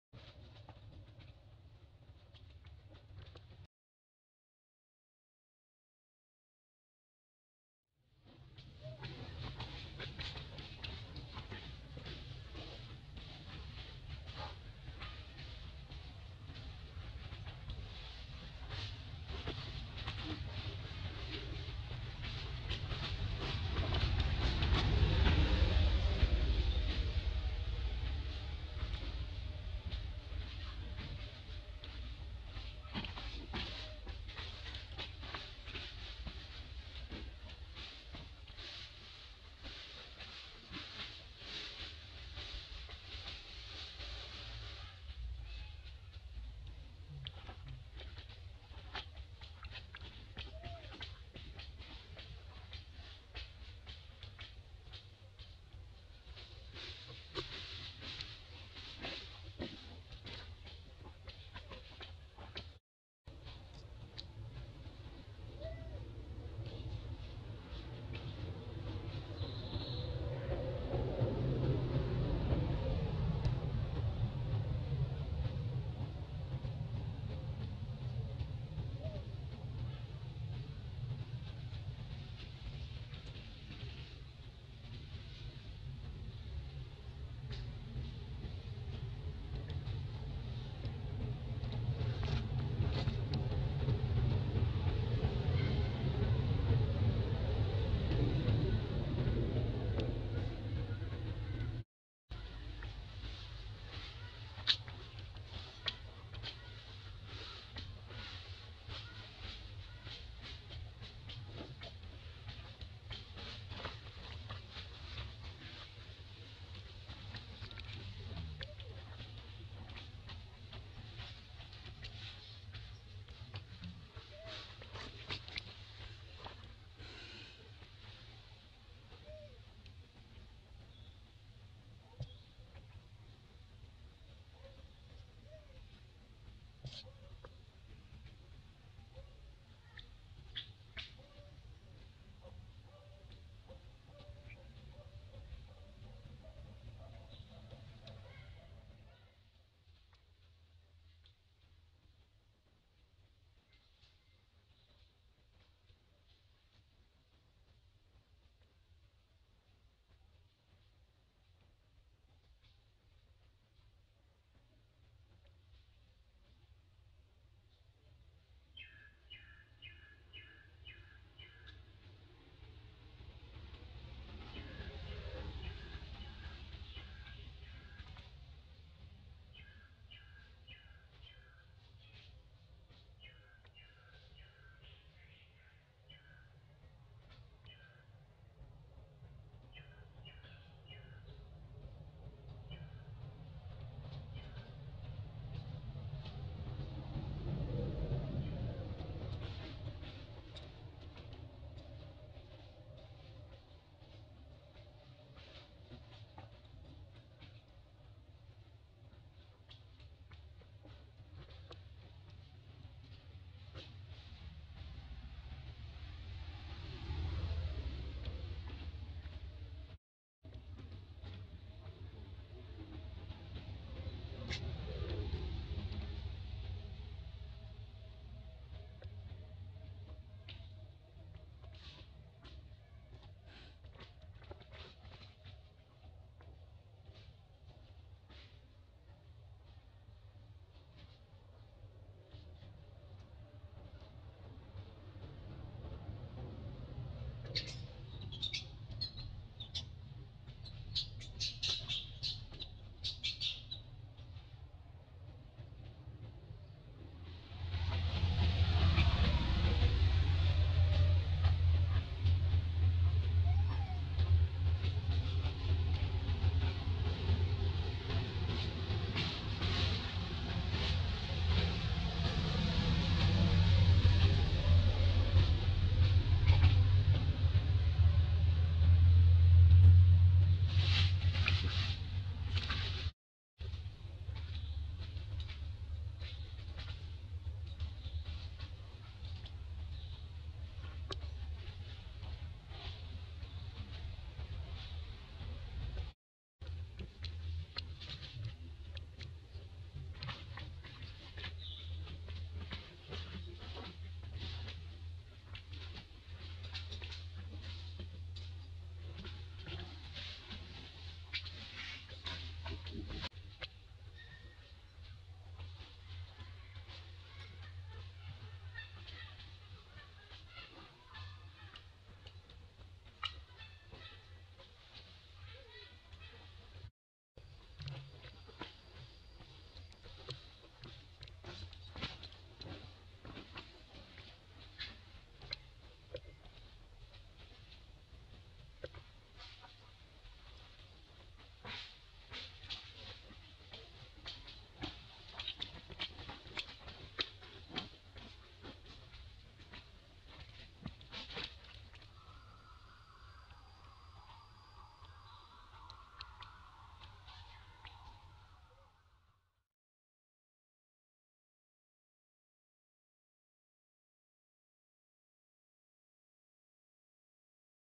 Poor Baby Anissa Was Very Healthy Heart & Body When Live With Father Aron, Father No Bad Like Anna,
Dear all Value Visitor, This Videos We Want To Show You About , Poor Baby Anissa Was Very Healthy Heart & Body When Live With Father Aron, Father No Bad Like Anna,